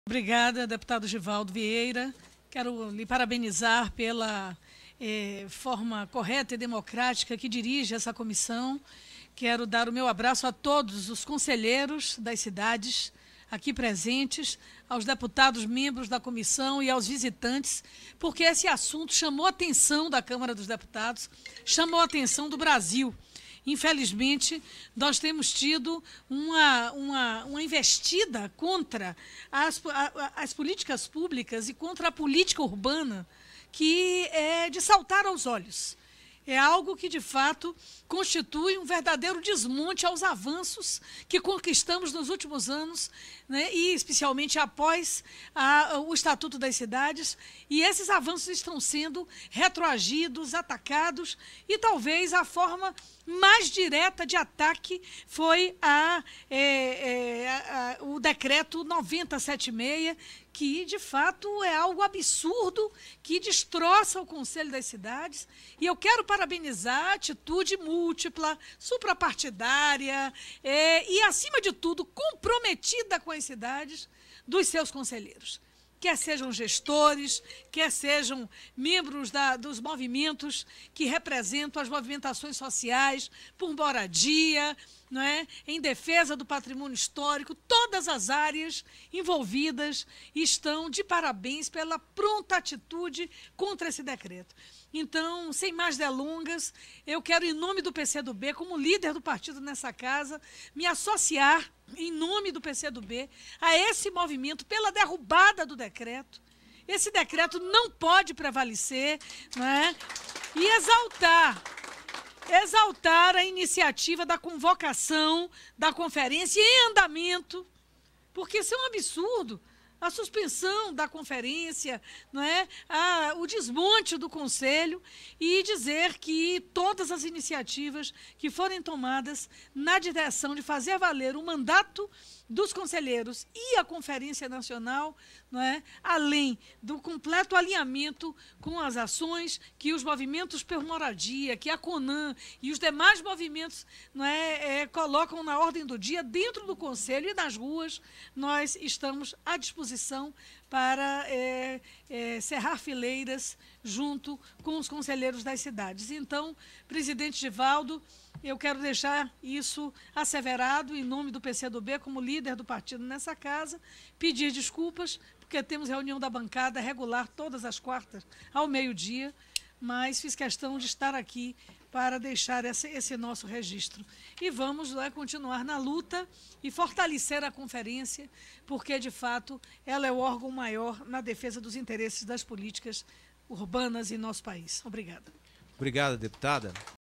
Alice Portugal critica ataque à gestão democrática das cidades
A deputada Alice Portugal, líder do PCdoB na Câmara, participou, no dia 28 de junho, de audiência pública na Comissão de Desenvolvimento Urbano (CDU) da Câmara, na qual ela é membro titular, que discutiu a "Gestão Democrática das Cidades" no âmbito da atuação do Ministério das Cidades. No evento, Alice denunciou o desmonte do Conselho Nacional das Cidades pelo governo Temer, que publicou o decreto 9.076/2017, retirando do Conselho o poder de convocar e organizar a Conferência Nacional das Cidades para este ano. O decreto adia a 6ª Conferência Nacional para 2019.